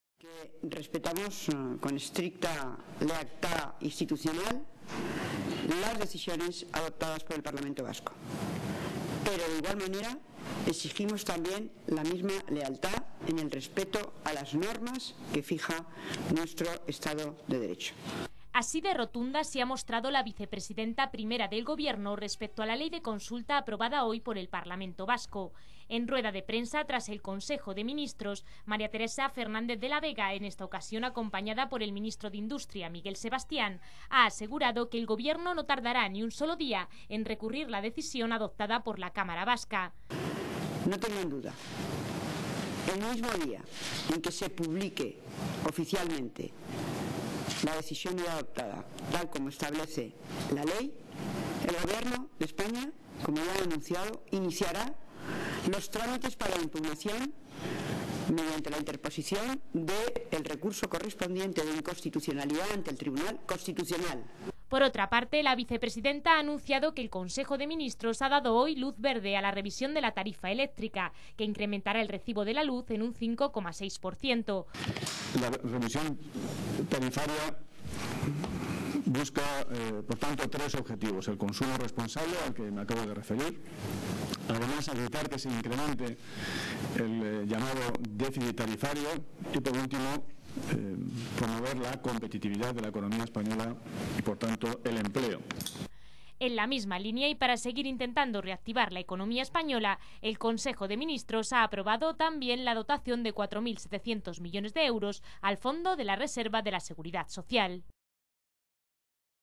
Recurrir ante el TC
La vicepresidenta primera del Gobierno, María Teresa Fernández de la Vega, afirmó hoy que el Ejecutivo "no tardará ni un sólo día en poner en marcha el mecanismo" para recurrir ante el Tribunal Constitucional la consulta aprobada en el Parlamento vasco con el apoyo de uno de los parlamentarios de PCTV, puesto que iniciará los trámites en el mismo momento de su publicación en el Boletín Oficial del País Vasco.